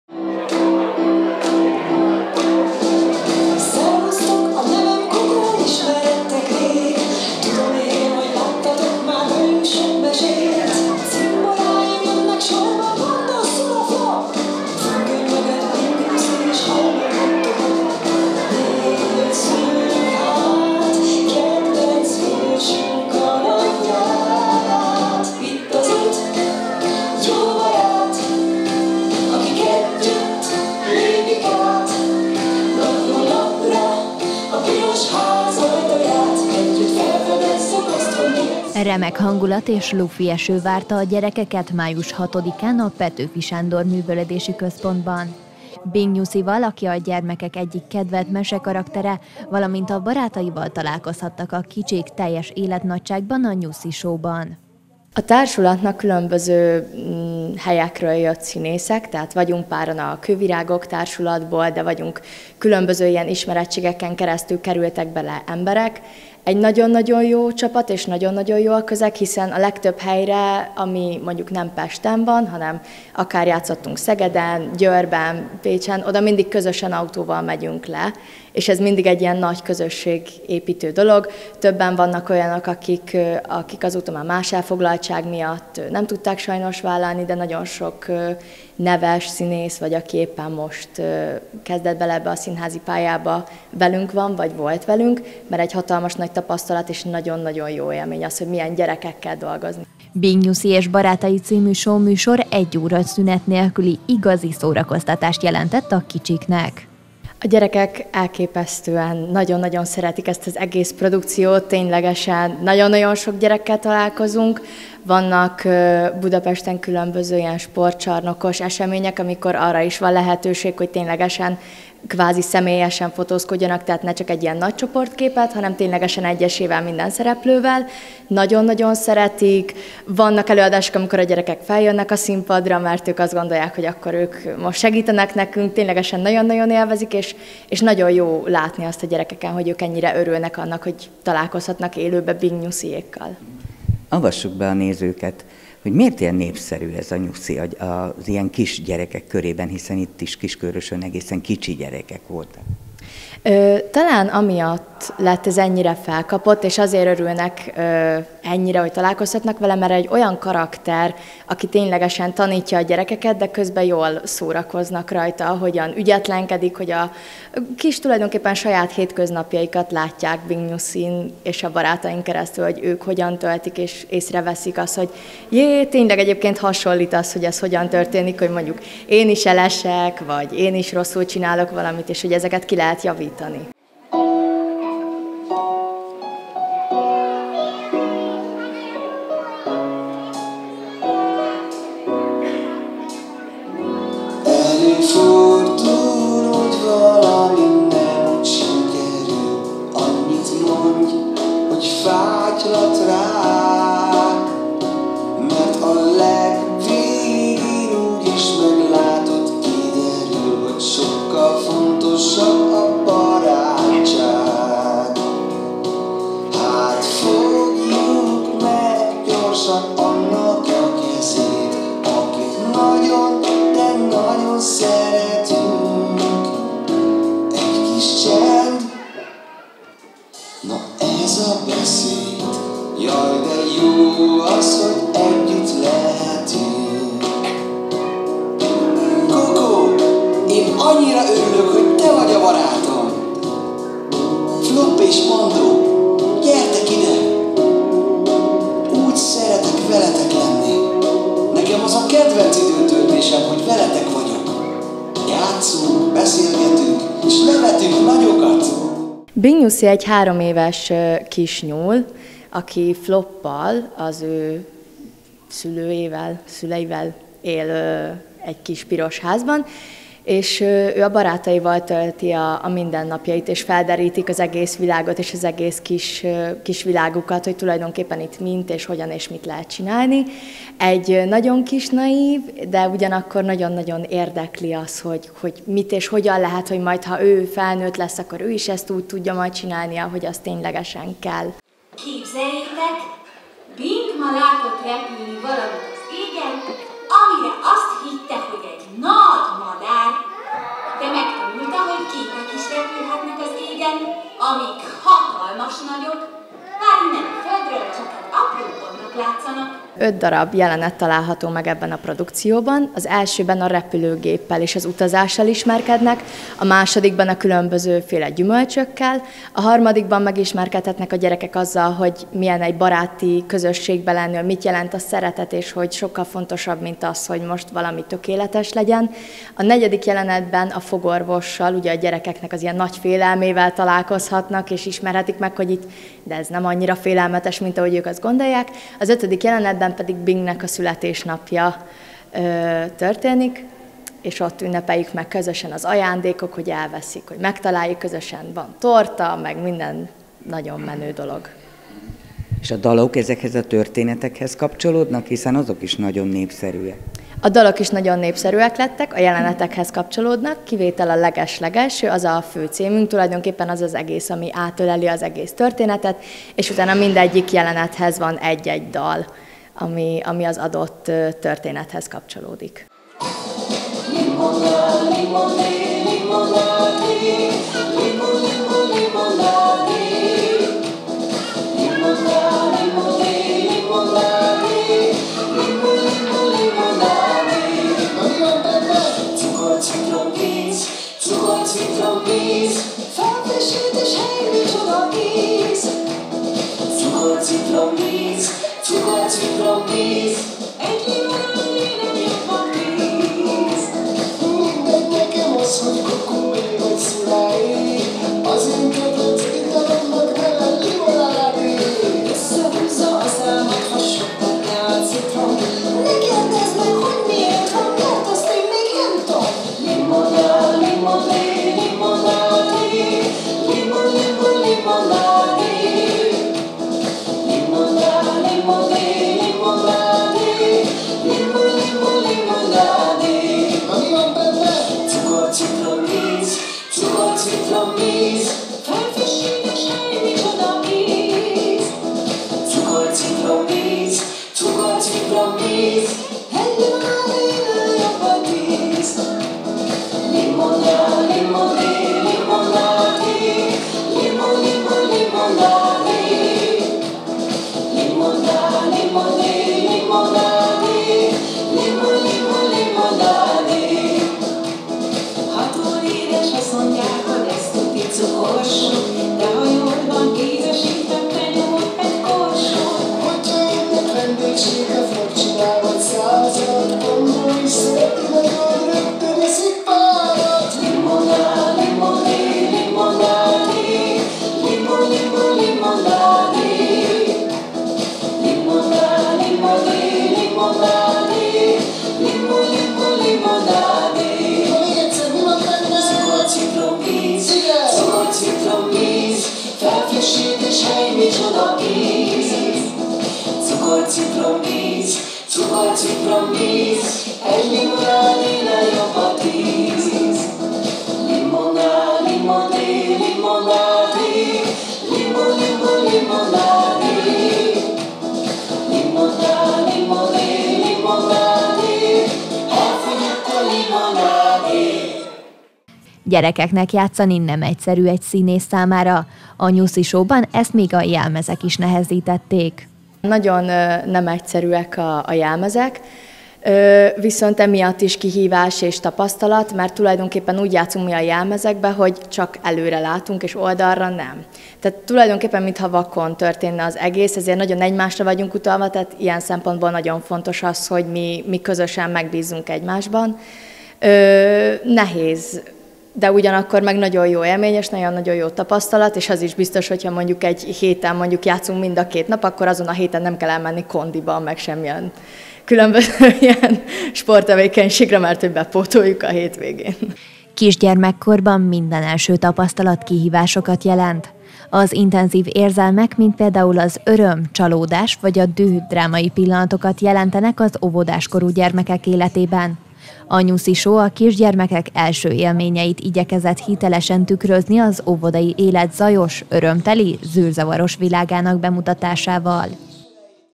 PETŐFI TÉR - Bing nyuszival és barátaival találkozhattak a gyerekek Kiskőrösön
A Petőfi Sándor Művelődési Központban egy fergeteges hangulatú show keretében találkozhattak a kiskőrösi gyerekek Binggel, a kedvenc mesekarakterükkel, valamint a barátaival: Cocoval, Szulával, Pandóval és Floppal május 6-án. Az előadás öt kis tanulságos történetet mesélt el, hat dallal színesítve.